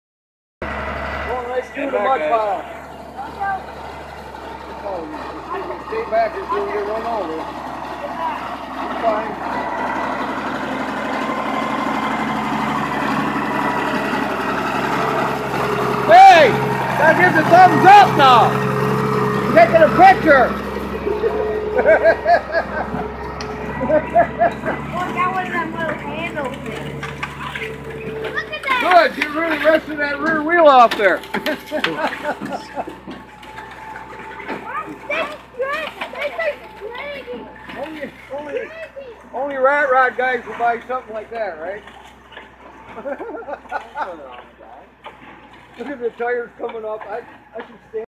1938 international bus
1938 international bus going to its new home.